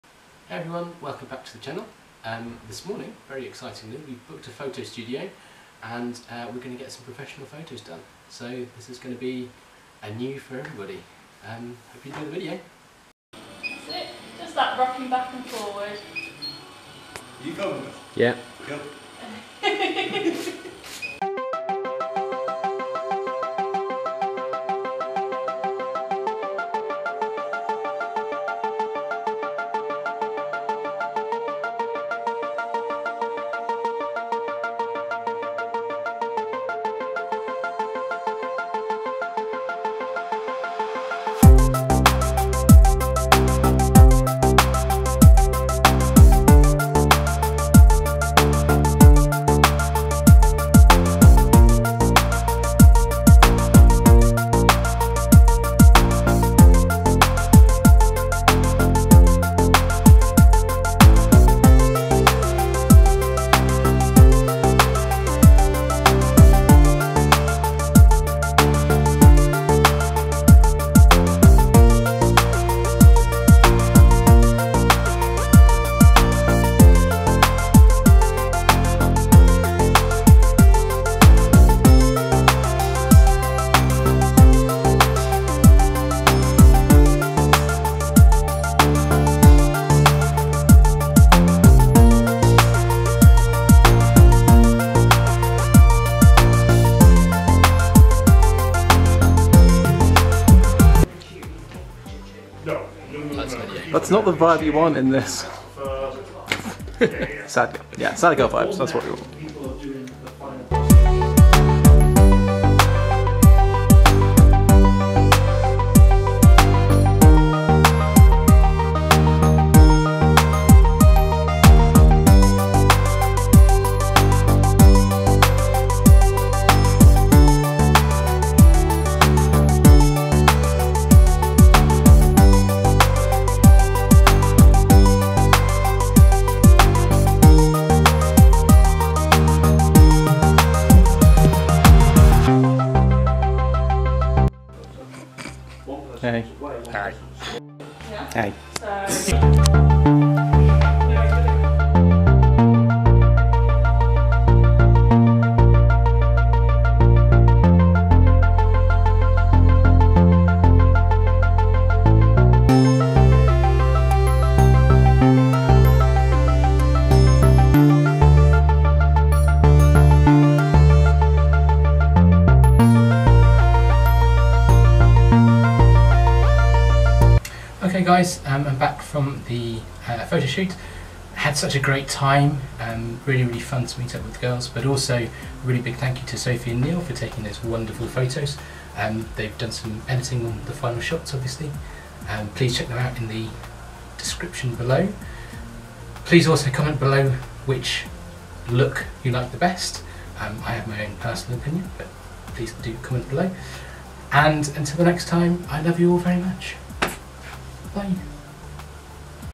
We Hired a Photo Studio! | Crossdress Life
Four girls and two professional photographers. Here's what we did!

Amazon Wish List